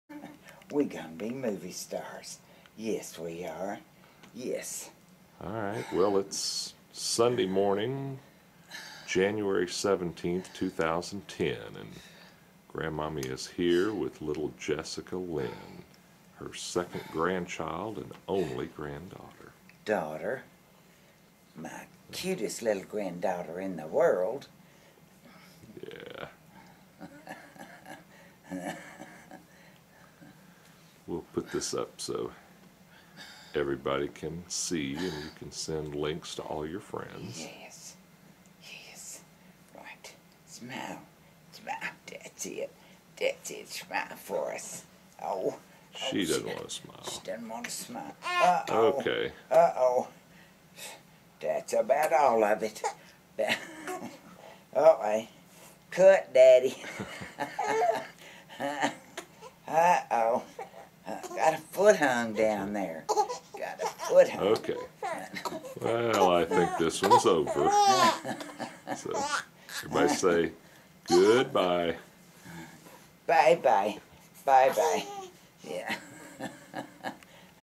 Jessica Tompkins & Grandmommy.MPG
Little Jessica Lynn Tompkins and her Grandmommy, Myrtle Tompkins, on Jan 17 2010.